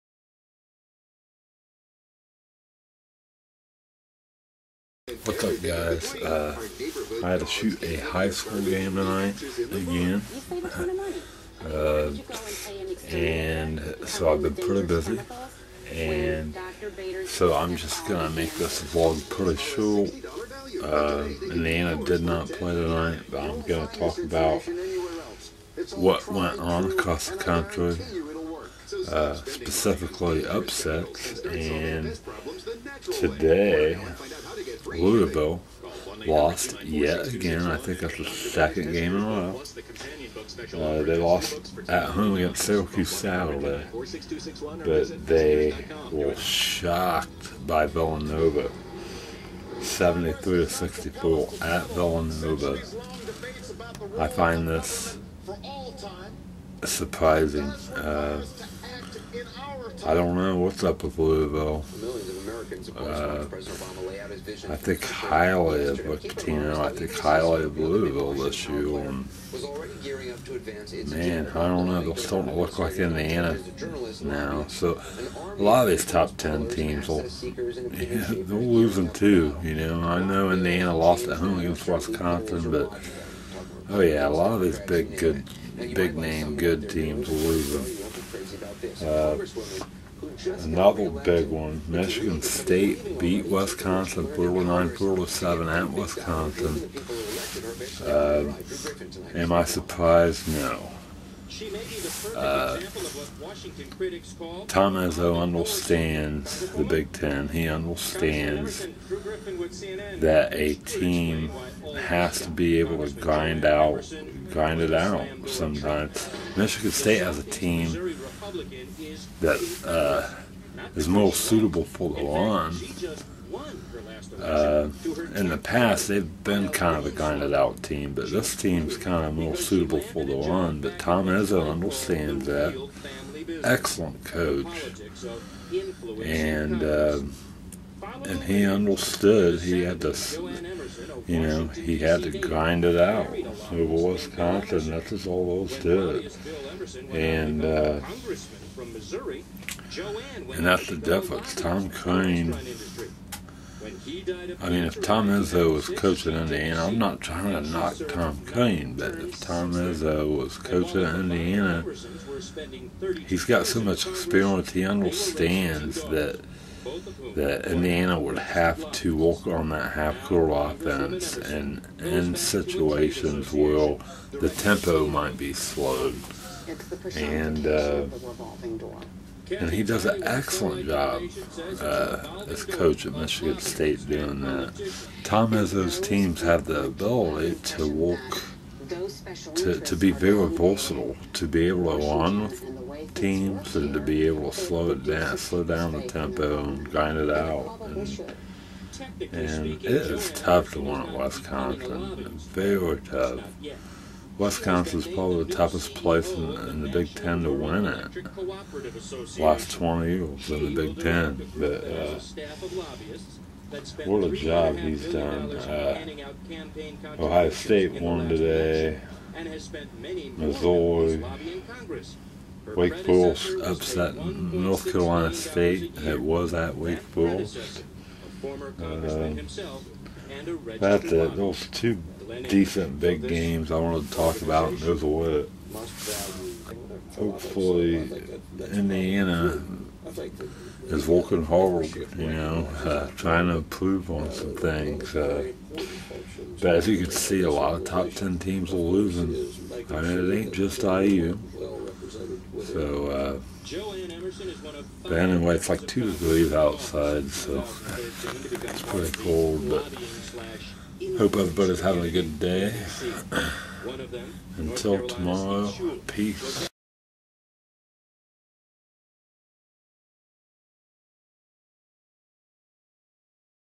"Lousiville Upset Again! What Is Going On?" 1-22-2013
One of my shortest vlogs EVER! I talk about Lousiville getting upset again to Villanova, and Michigan St. winning at Wisconsin!